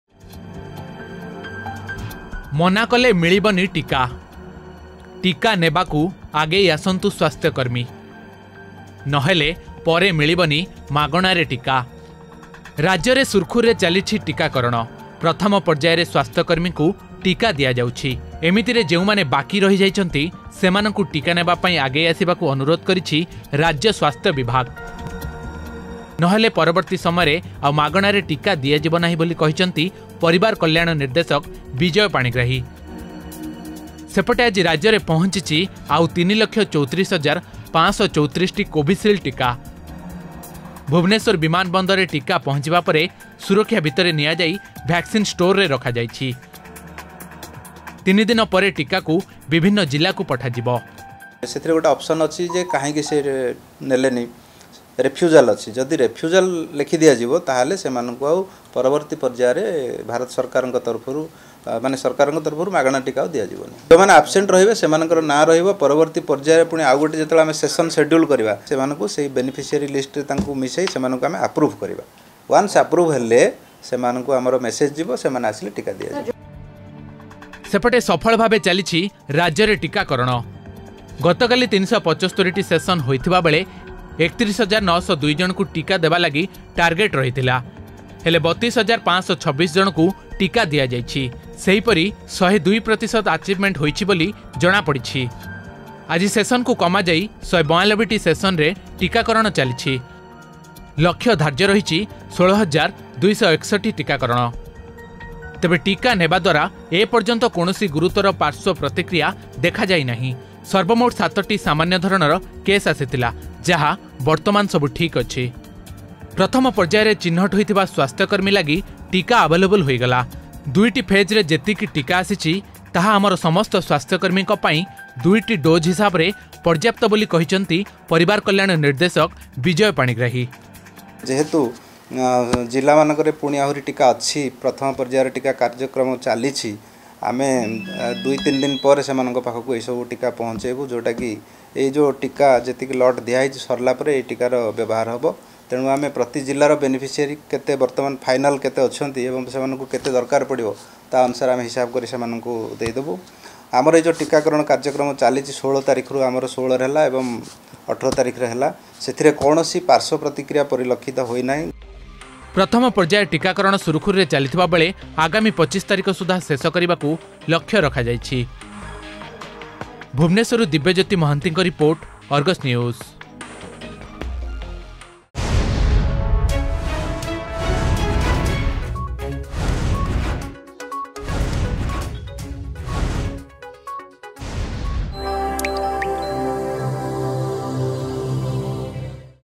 ମନାକଲେ ମିଳିବନି ଟିକା..
ମନାକଲେ ମିଳିବନି ଟିକା ,ଟିକା ନେବାକୁ ସ୍ୱାସ୍ଥ୍ୟକର୍ମୀ ଆଗେଇ ଆସିବାକୁ ଅନୁରୋଧ I ନହେଲେ ପରେ ମିଳିବନି ମାଗଣାରେ ଟିକା I ଋାଜ୍ୟ ସ୍ୱାସ୍ଥ୍ୟ ବିଭାଗର ସୂଚନା I ରାଜ୍ୟରେ ପହଞ୍ଚିଛି ଆଉ ୩ ଲକ୍ଷ ୩୪ ହଜାର ୫୩୪ କୋଭିସିଲ୍ଡ ଟିକା  I ତିନି ଦିନ ପରେ ଟିକାକୁ ବିଭିନ୍ନ ଜିଲ୍ଲାକୁ ପଠାଯିବ ...